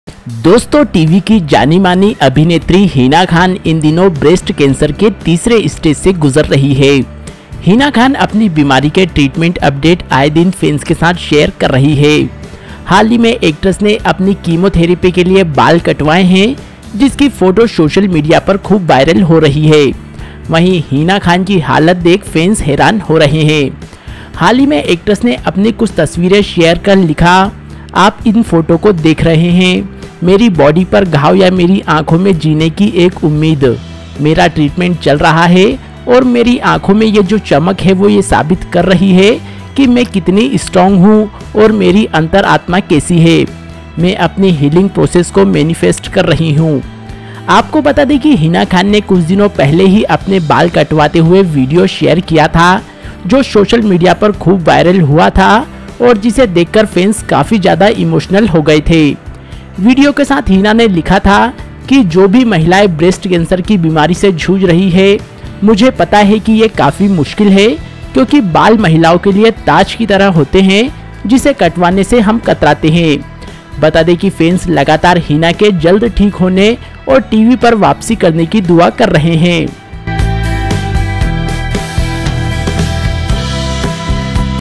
एक्ट्रेस Hina Khan की हालत हुई ख़राब, शरीर पर बने ऐसे निशान देखकर फैंस मांग रहे दुआ
TV actress Hina Khan is currently undergoing the third stage of breast cancer. Hina Khan is sharing treatment updates of her illness with fans every day. Recently, the actress has cut her hair for her chemotherapy, the photo of which went viral on social media! Fans are shocked to see the condition of Hina Khan